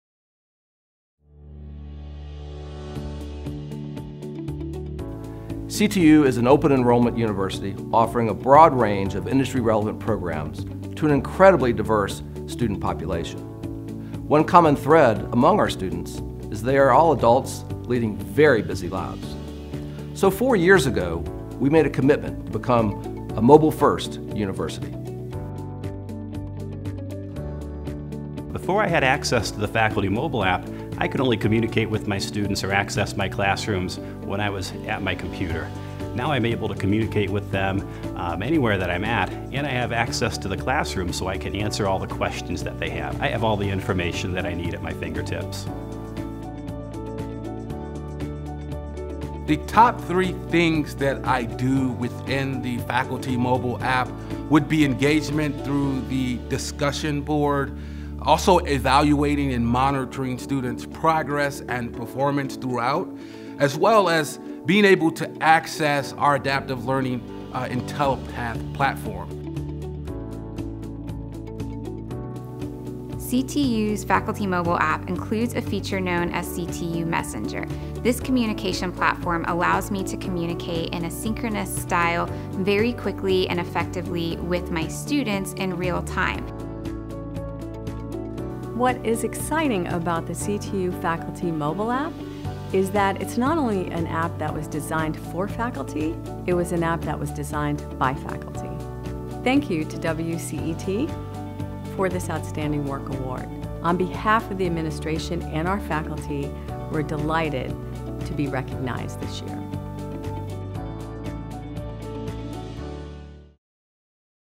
CTU Faculty App 2018 WOW Award | Colorado Technical University
Our university was proudly named a 2018 WCET Outstand Work (WOW) Award winner for the CTU Faculty App. With the app, online and campus instructors can engage with their courses and connect with students directly from their mobile devices. Learn more about CTU's awards for mobile innovation